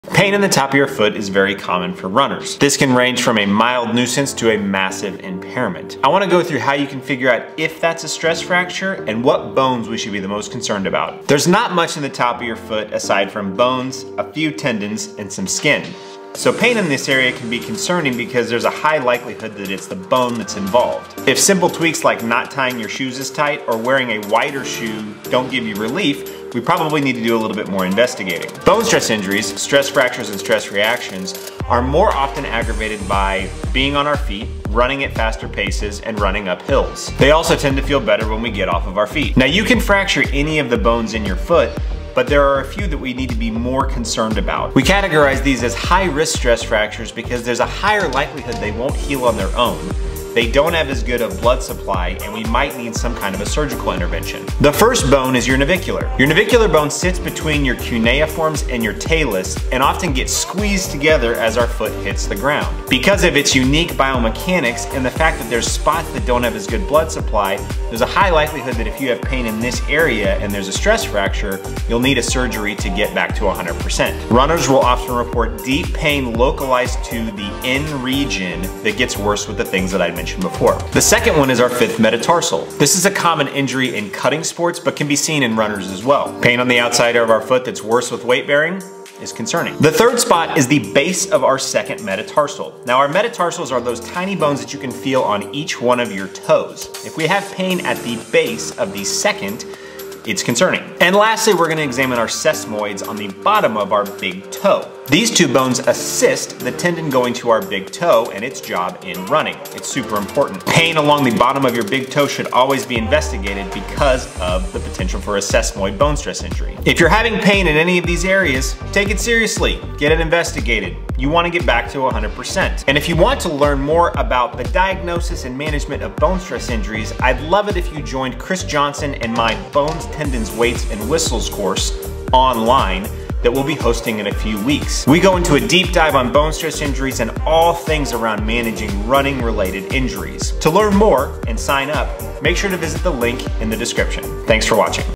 High Risk Stress Fractures | Locations, Diagnosis, & Treatment
Is this a stress fracture? 00:28
What are High-Risk Stress Fractures? 00:44
What Bones in the Foot are High Risk? 00:54

Pain on the top of your foot is common in runners and can range from a mild nuisance to a massive impairment. I want to go through how to tell if you might have a stress fracture and what foot bones we should be the most concerned about.

There's not much on the top of your foot besides bones, some tendons, and skin. Pain here should be concerning because of the high likelihood the bone is involved. If simple tweaks like not tying your shoes super tight and getting in a wider shoe don't help, we got to start thinking this might be a bone stress injury. Stress fractures/reactions tend to worsen when you are on your feet, running up hills, and running at faster paces. They also tend to feel better when you rest and get off your feet.
You can fracture any of these bones in your foot, but some are more serious than others. We classify as high-risk and should be managed quickly and appropriately.

Higher likelihood of it not healing because of the specific stress going through the bone.
Worse blood flow to the area.
Some bones have better blood supply. Higher risk of needing surgery to get back to 100%

Tying your shoes too tight
Leave a gap over the painful area.
Gradual onset of pain that stays around for more than a few runs. Worse with weight bearing, faster running, and managing inclines.

High Risk

Navicular
Proximal Diaphysis of the 5th MT
Base of the Second MT
Great Toe Sesamoids

Navicular Bone

It can get squeezed between its neighbors, the talus and the cuneiforms.
Deep, located pain with running. You'll typically be tender right over the bone called the "N spot)

Base of the 2nd MT

Swelling and tenderness. Runners and dancers

Great Toe Sesamoids

Repetitive stress overload under your first metatarsal

Take pain in these areas seriously! Get them checked out early so you can get the best management plan in place. Suppose you want to learn more about examining runners for potential BSIs and build a comprehensive rehab plan. In that case, I'd love to have you join Chris Johnson and my Bones, Tendons, Weights, and Whistles Course on April 7th.